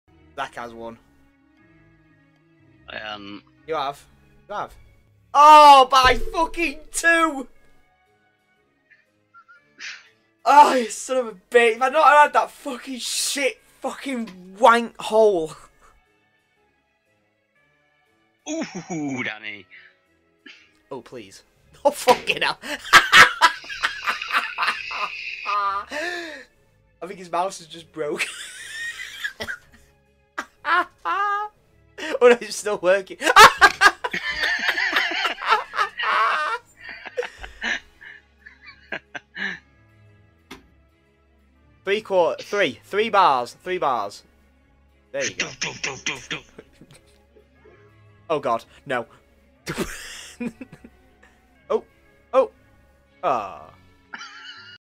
We have our first mouse rage! :D DANNYYYYYY
So as we are recording a lot of games to then sit and get a few good videos on our channel. heres something we caught during one of our mini golf games! :D RIP dannys mouse